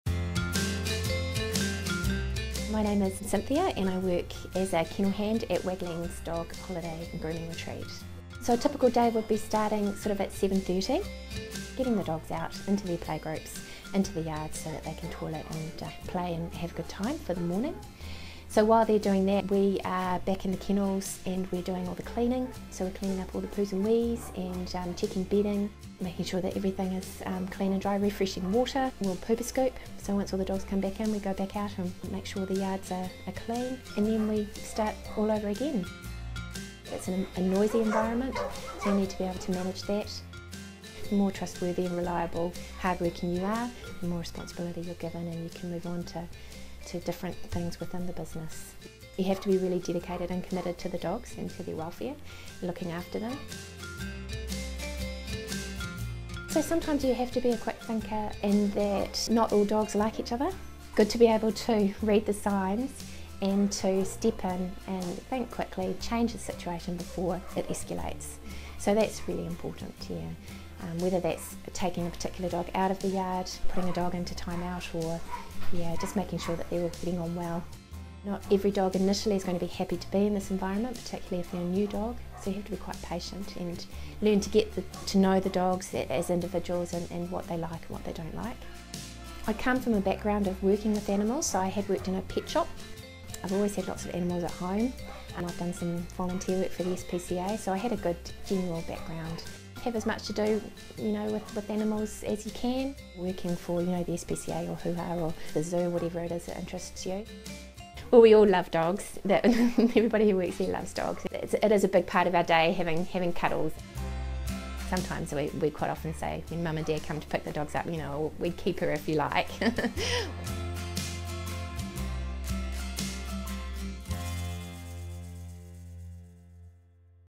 Animal Care Attendant - A day in the life
A day in the life of an animal care attendant, Cynthia Knight.